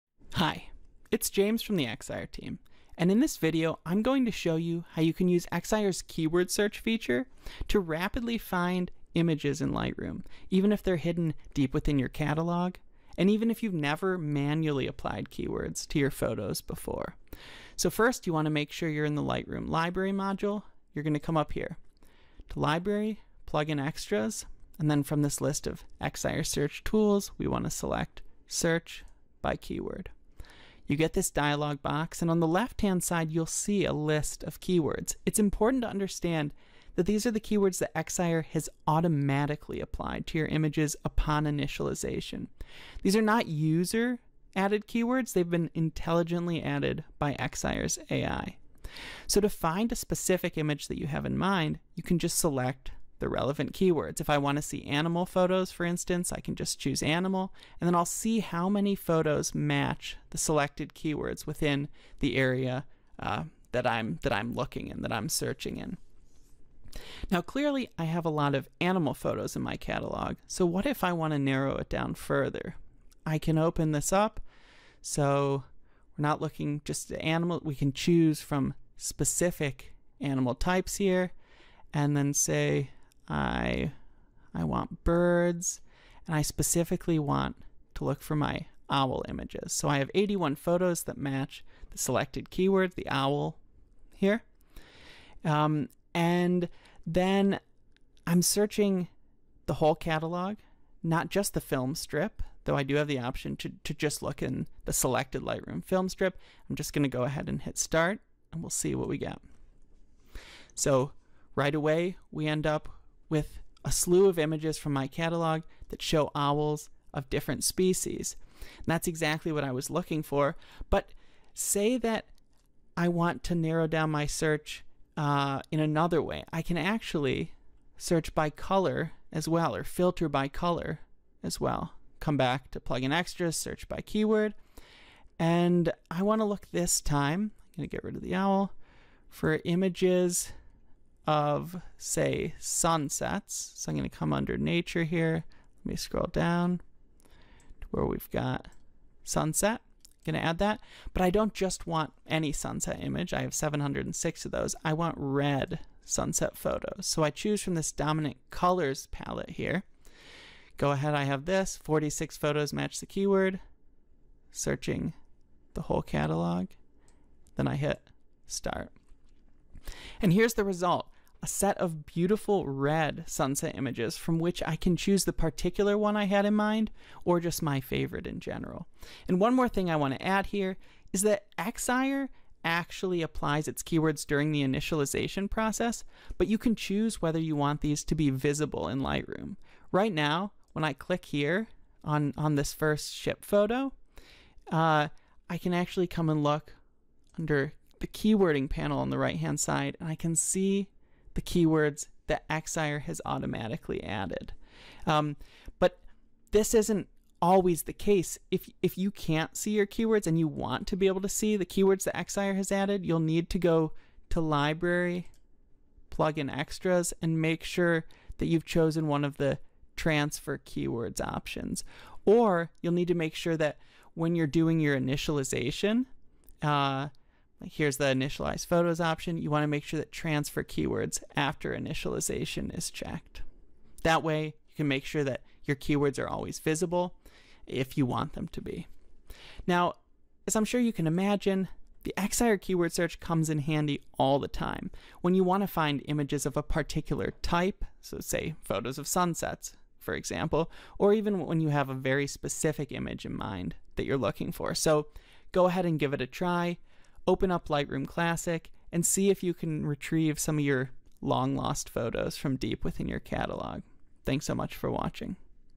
How to Find Photos in Lightroom With the Search By Keyword Tool [Excire Search 2024 Plugin]
Rapidly find photos in your Lightroom catalog using Excire's Search by Keyword tool!

Excire's Lightroom plugin automatically adds keywords to your photos - and helps you find them!

In this short tutorial, discover how to use the Search by Keyword tool to retrieve images in Lightroom using Excire's AI-generated keywords.

Learn about Excire's AI-powered Lightroom plugin, Excire Search 2024, right